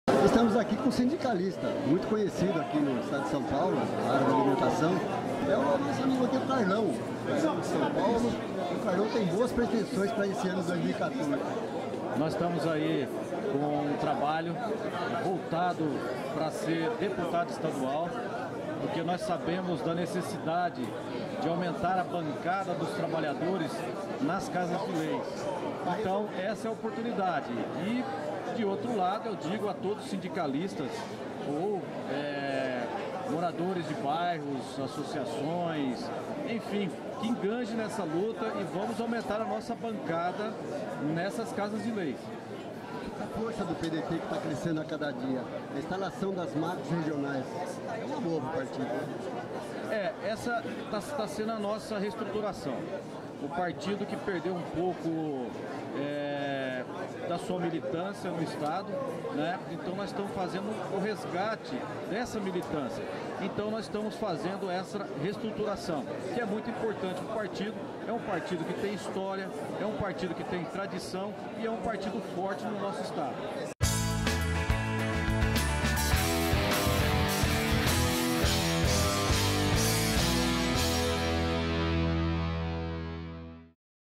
Carlão do Sindicato da Alimentação é pré-candidato a deputado
A revelação foi feita no sábado durante encontro do PDT na cidade de Osasco onde foi debatida a reestruturação o partido em São Paulo e região.